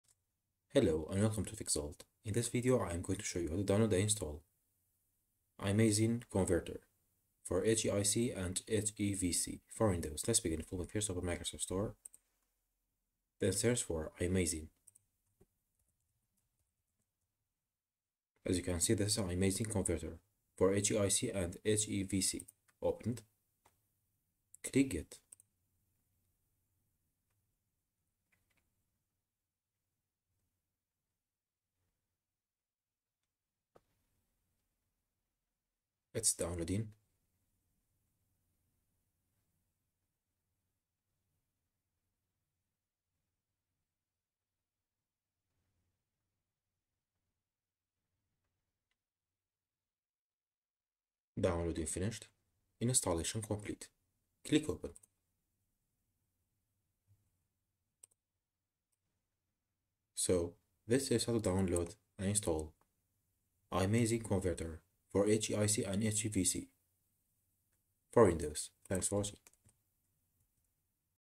How to Download and Install iMazing Converter for HEIC and HEVC For Windows
iMazing Converter is the first 100% free solution to easily convert photos and video taken in the new HEIF/HEIC and HEVC formats. Easy to use, efficient, it's a cute little utility we hope you'll find useful!

Features:
- Convert HEIF/HEIC files to JPEG or PNG
- Convert HEVC (H.265) files to MPEG-4 (H.264)
- Adjustable compression/quality and resolution of JPEG or MPEG-4 output
- Preserve EXIF data when converting to JPEG
- Super easy to use: drag and drop files or folders and hit Convert
- Available on macOS too!